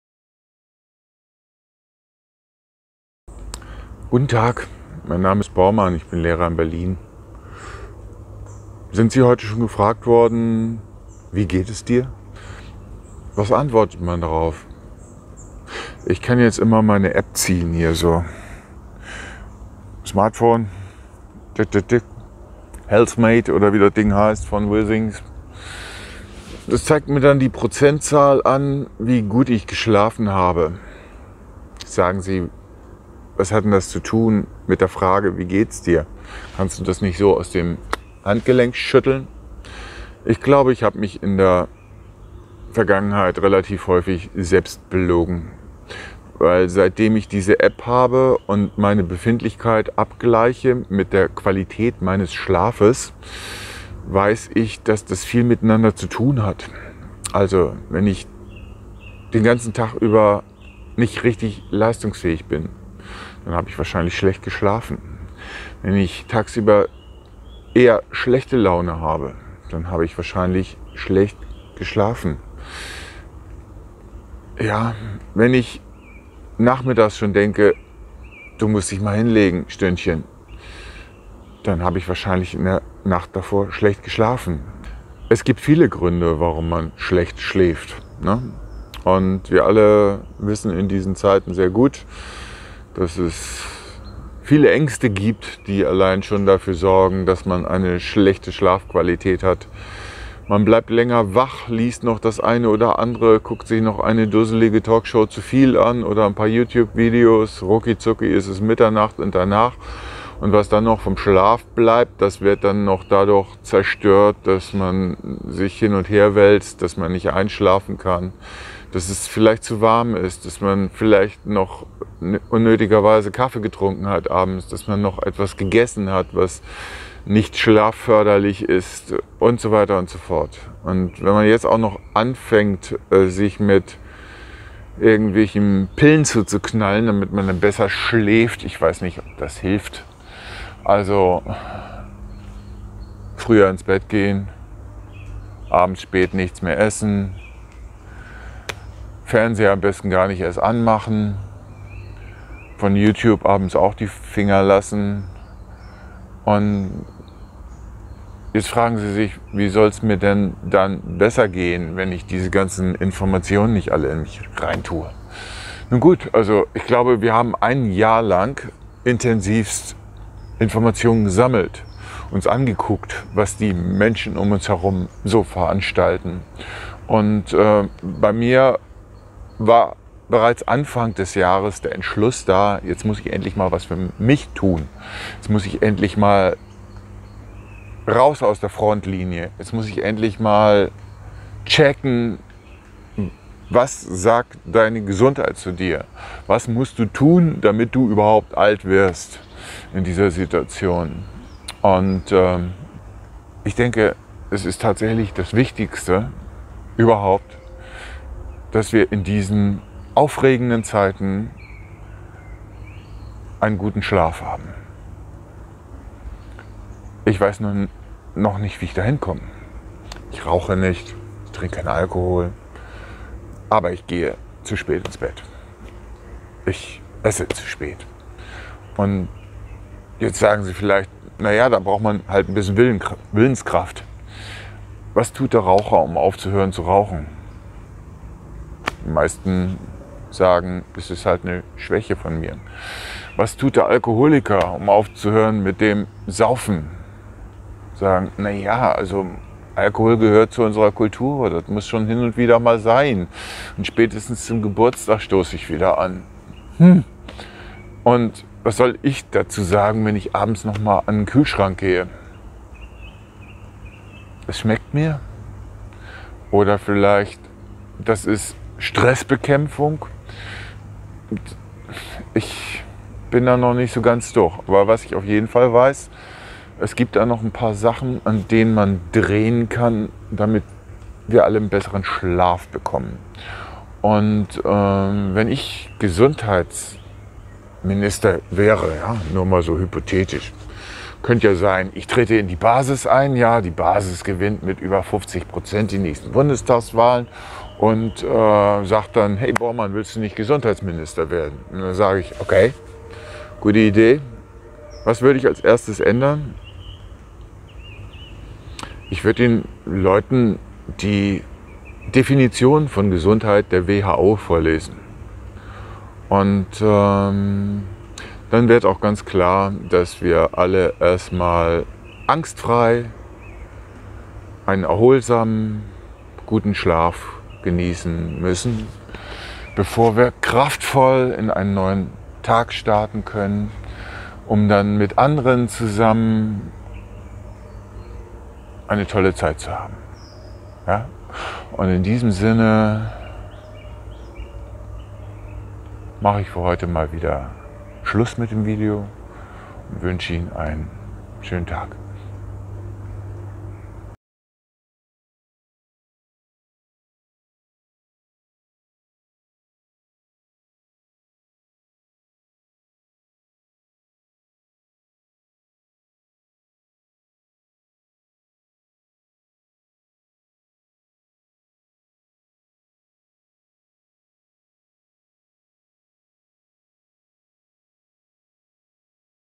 Gut geschlafen? Ich leider mal wieder nicht ...
Gesund bleiben ist schwierig in einer Zeit, in der alles, was wir in unserem Leben gelernt haben, von sogenannten "Experten" auf den Kopf gestellt wird. Es wird Zeit, dass die Verantwortlichen in dieser "Gesundheitskrise" eine Quittung bekommen. Wir kümmern uns jetzt selbst um unsere Gesundheit! Wir sparen uns die Gehirnwäsche am Abend, machen den Fernseher früher aus und schlafen uns gesund. :-)